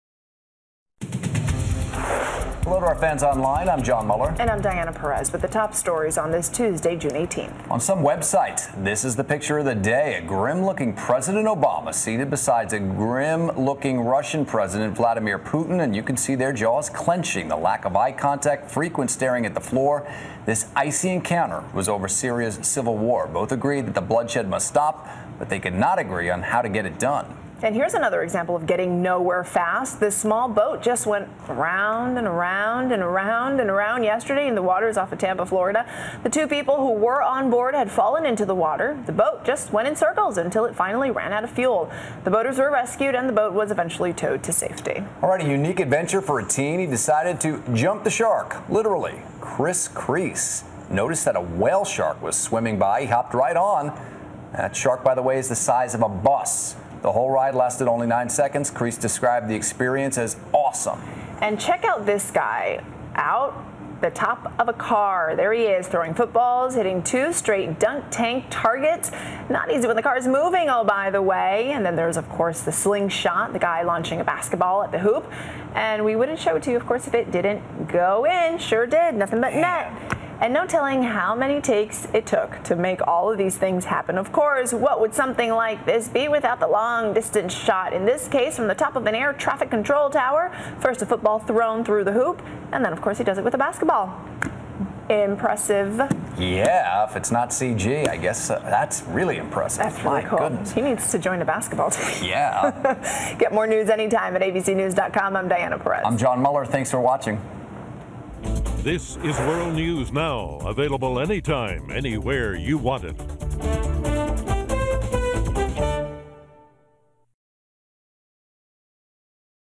WEBCAST Perfect Aim!
Also, the picture that's making it rounds on some websites showing President Obama and Russian President Vladimir Putin sharing a grim moment while recently meeting each other.